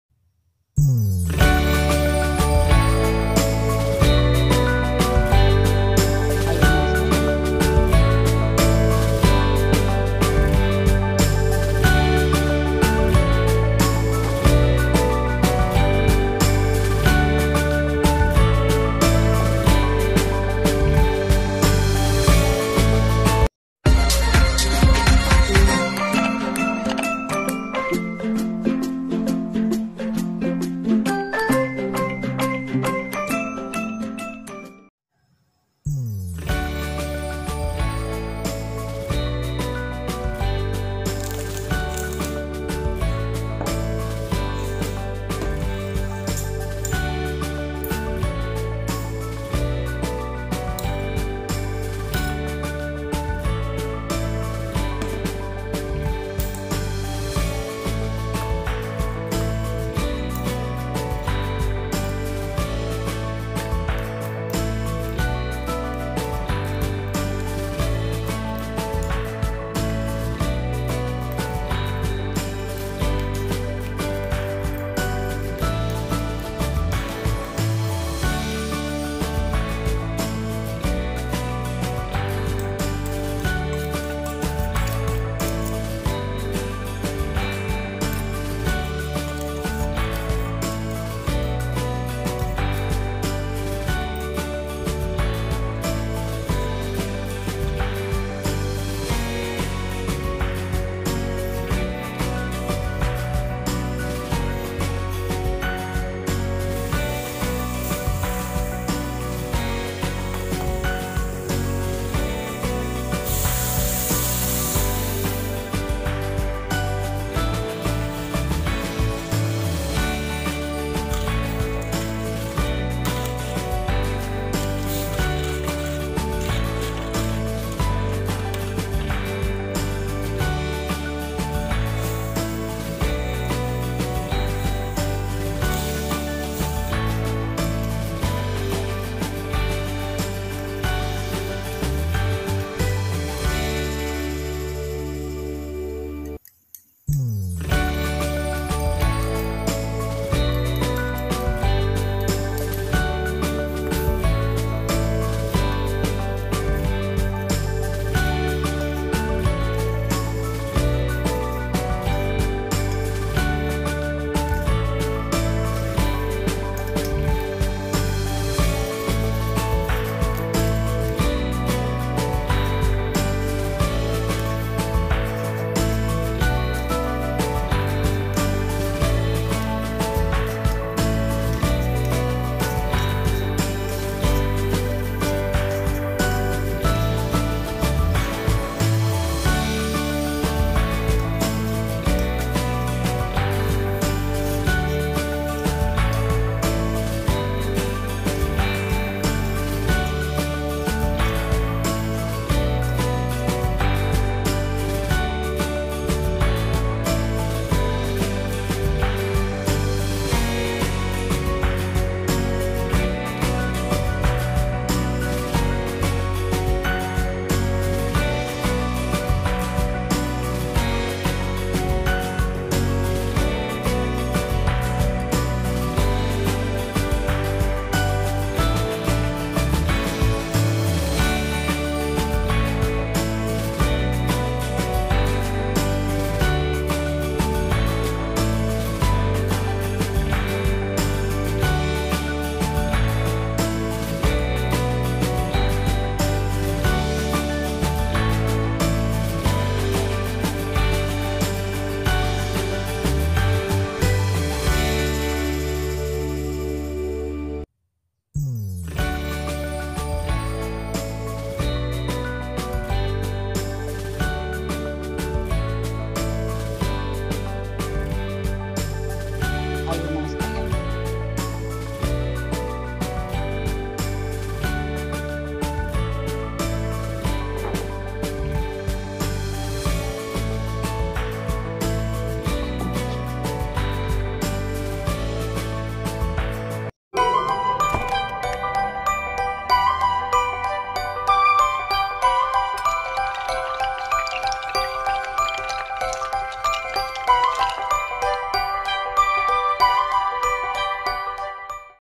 Cara Membuat Permen Asam Jawa
bahan bahan
tepung panir/tepung roti
gula merah
gula pasir
air  panas
air
jahe